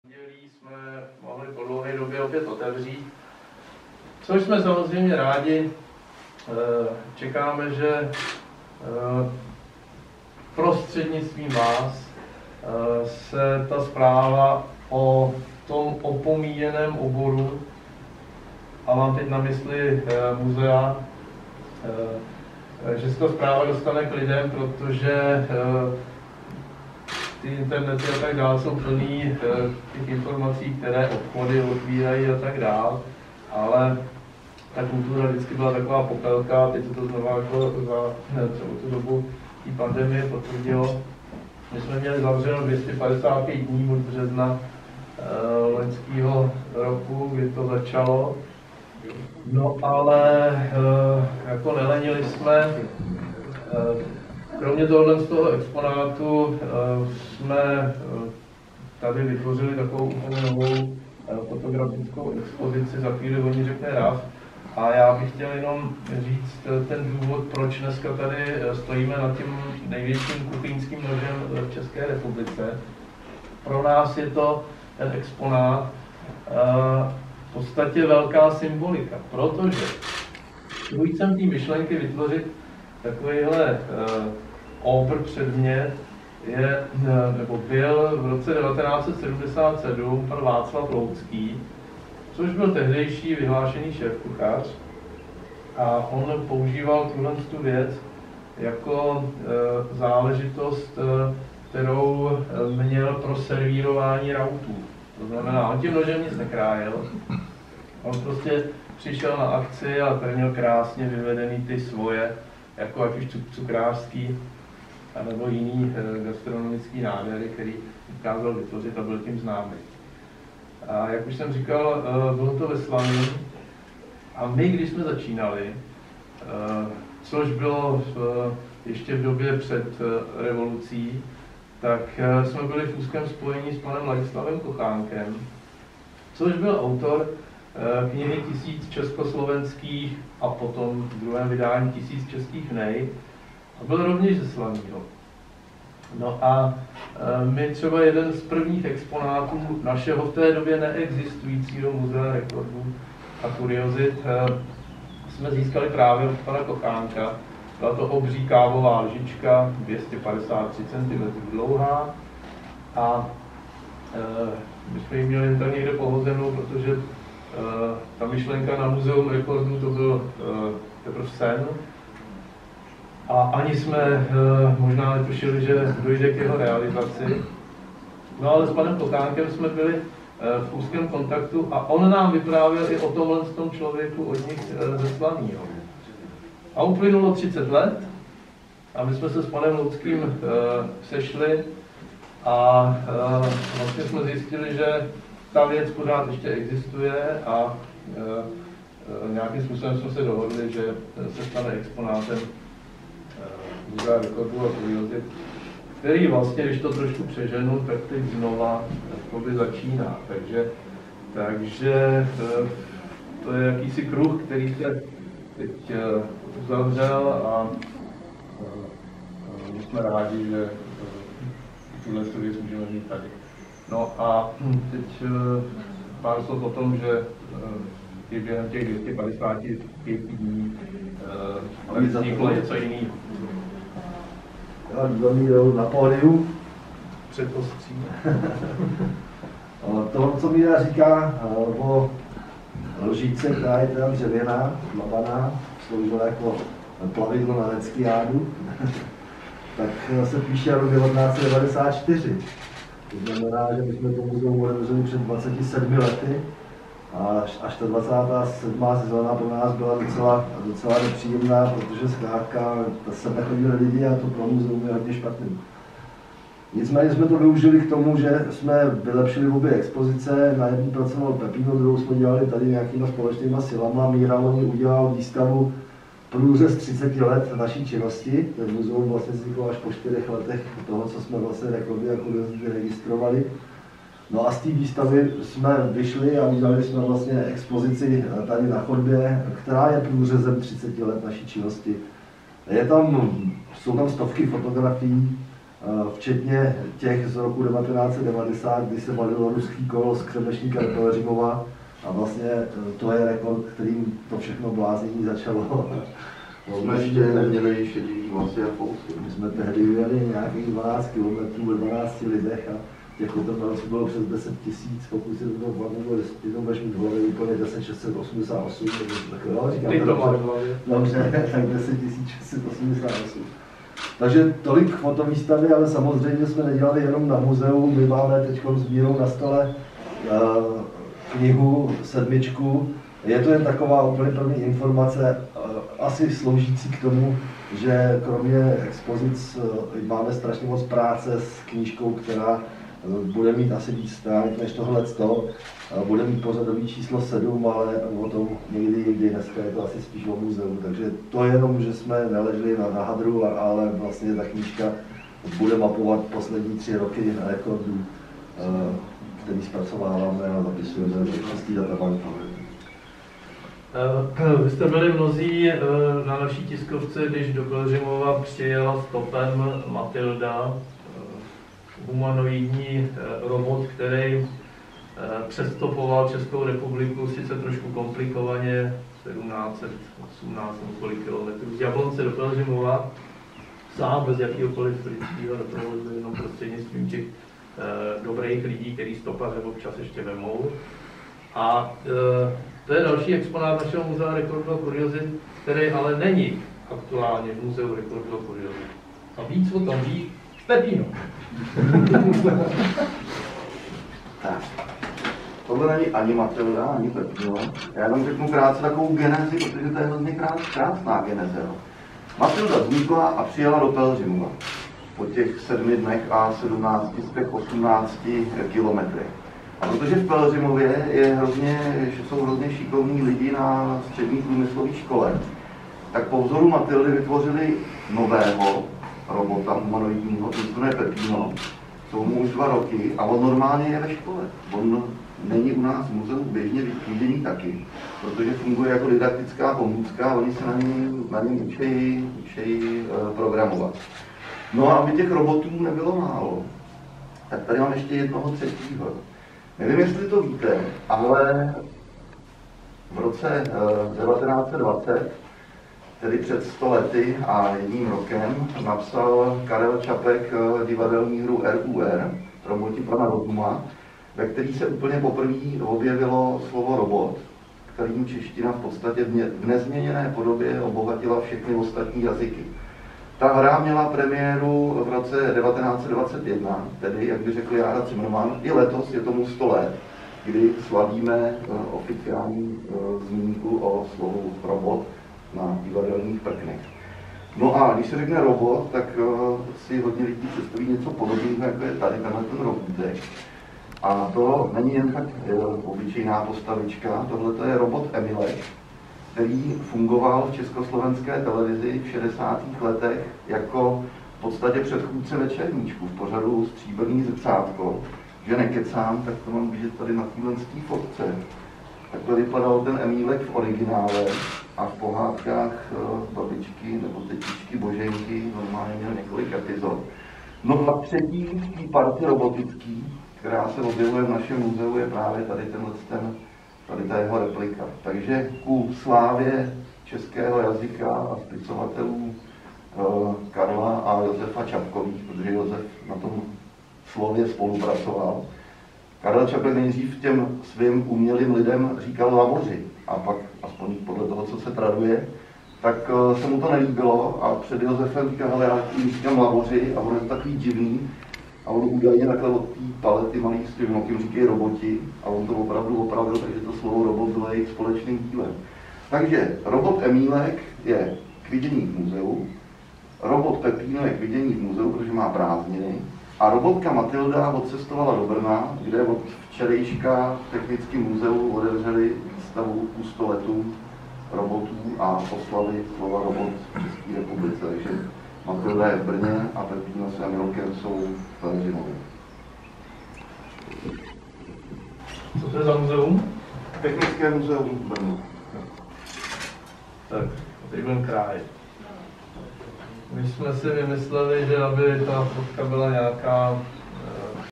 Největší nůž - tiskovka
Od května je v Muzeum rekordů a kuriozit v Pelhřimově nový pozoruhodný exponát: Největší kuchyňský nůž. Délka nože je 286 cm a v nejširším místě měří 54 cm. Natočeno 13. května 2021.